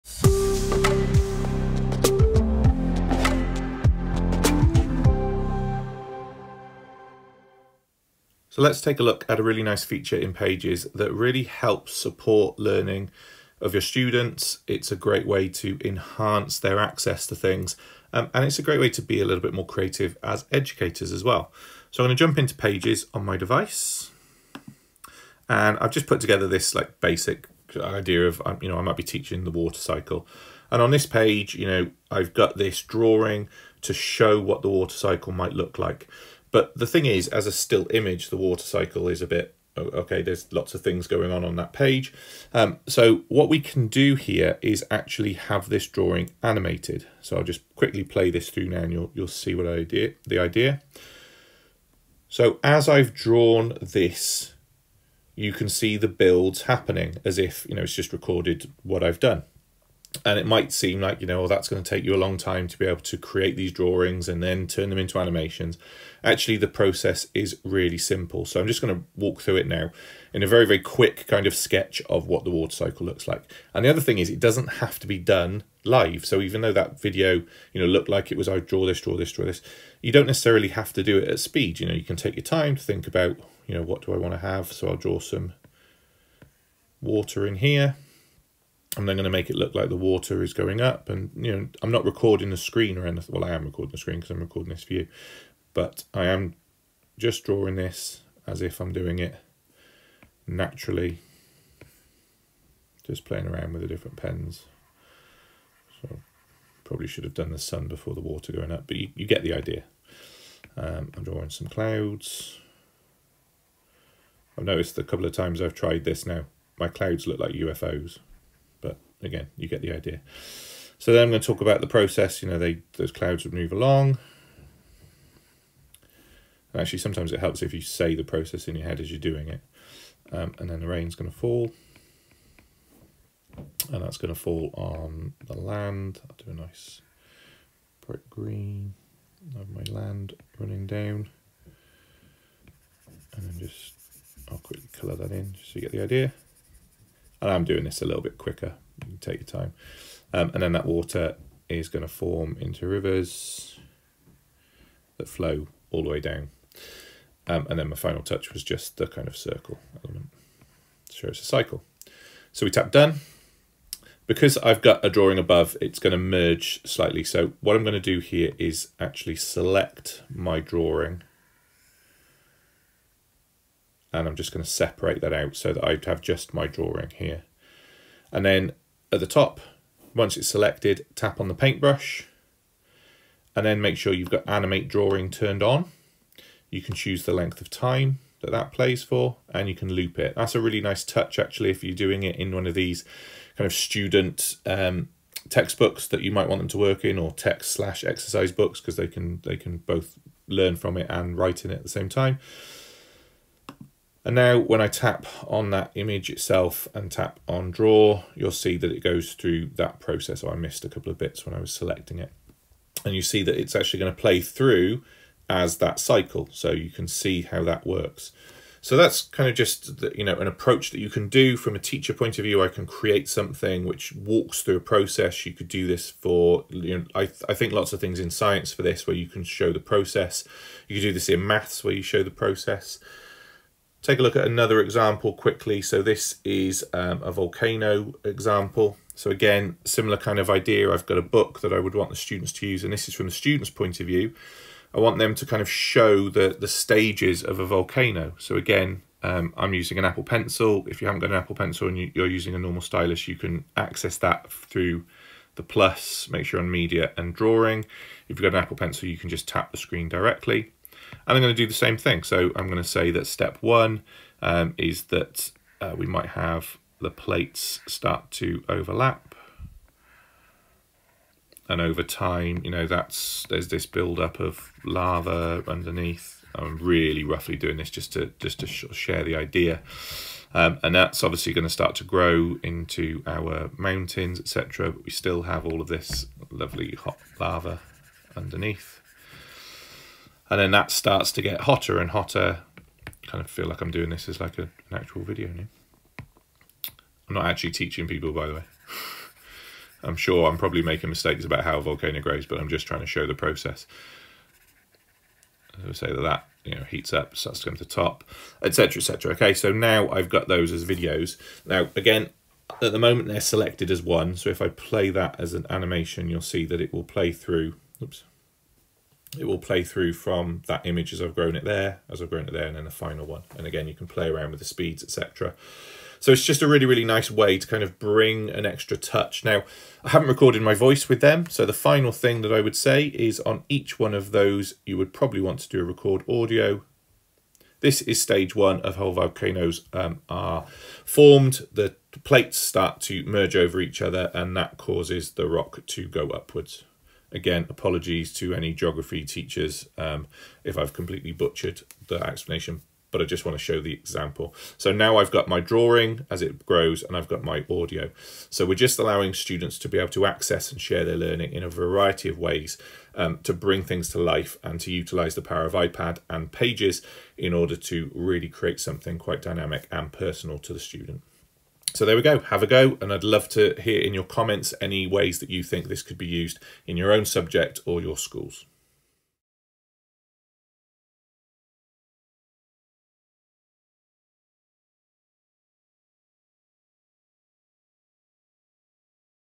Pages Tips: Using Animation to support learning (iPad Tutorial 2020)
Let's take a look at how the iPad Pages app allows you to be creative with drawing and really bring new opportunities to learning and teaching to support students.
The drawing tool alone is a great way to allow access, but turning them into instant animations, really adds a new dimension.